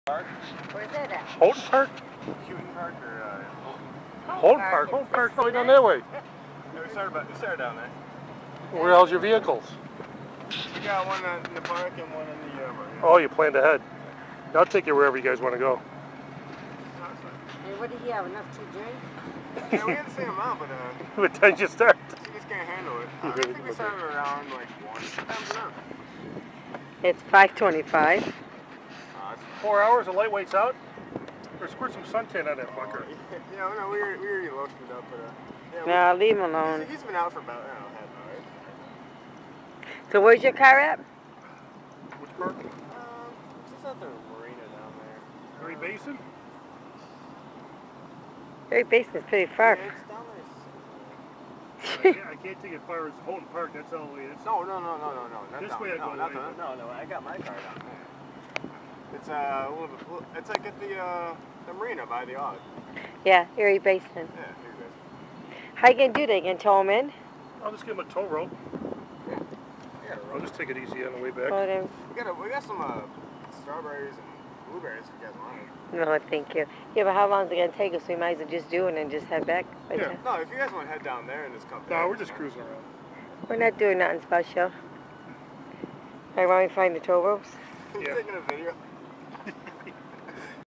Drunk kids floating in Buffalo River
Ran into a couple of kids from E. Amherst who planned their white water rafting trip down the Buffalo River from Google Maps.